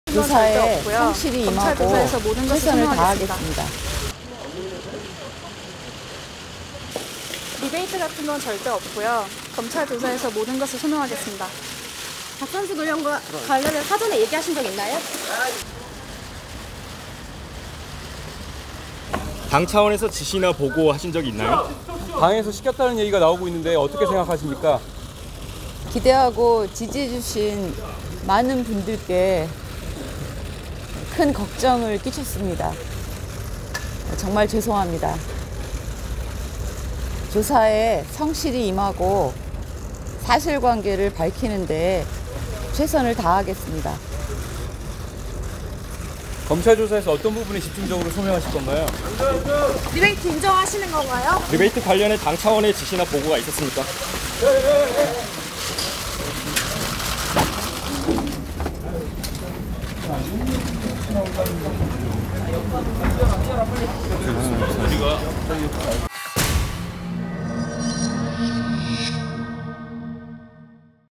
박선숙 의원 '총선 리베이트 의혹' 검찰 출석
'국민의당 총선 홍보비 리베이트 의혹'과 관련, 검찰에 소환된 박선숙 국민의당 의원이 "지지해주신 많은 분들께 죄송하다"고 전했다.

27일 오전 9시 57분쯤 박 의원은 흰색 블라우스에 베이지색 정장을 입고 변호사를 대동한 채 서울 서부지검에 출두했다.

박 의원은 "기대하고 지지해주신 많은 분들께 큰 걱정을 끼쳐 정말 죄송하다"며 "조사에 성실히 임하고 사실관계를 밝히는데 최선을 다하겠다"고 밝혔다.

"당 차원에서 지시한 게 맞는지", "리베이트 전 당에게 보고가 있었는지"에 대한 취재진의 질문엔 답하지 않고 청사로 들어갔다.

앞서 지난 8일 중앙선거관리위원회는 지난 20대 총선 당시 사무총장이던 박 의원과 왕주현 사무부총장의 지시로 홍보업체가 조성한 불법 정치자금이 브랜드호텔을 통해 국민의당에 흘러들어갔다며 김 의원과 박 의원, 왕 사무부총장 등을 검찰에 고발했다.

선관위의 고발로 수사에 착수한 검찰은 지난 16일 왕 부총장을, 23일에는 김 의원을, 27일에는 박 의원을 차례로 소환했다.

한편, 구속영장이 청구된 왕 사무부총장이 이날 오전 10시30분부터 열리는 구속전피의자심문(영장실질심사)을 위해 서울서부지법에 출석했다.

왕 부총장은 이날 10시15분쯤 정문 앞에서 기다리고 있는 취재진의 눈을 피해 옆문을 통해 법원 건물로 들어갔다.